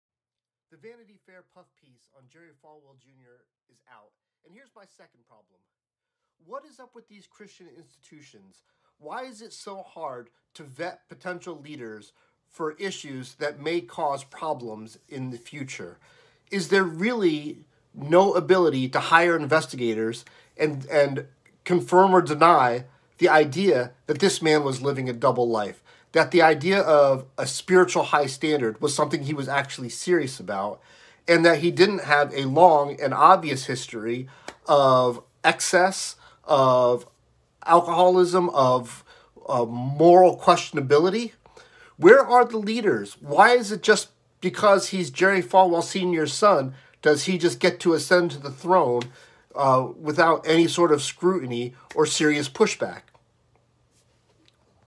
Jerry Falwell Jr Blames You! 2 Vanity Fair
Where was the leadership at Liberty? Was there no one who knew he was leading a double life?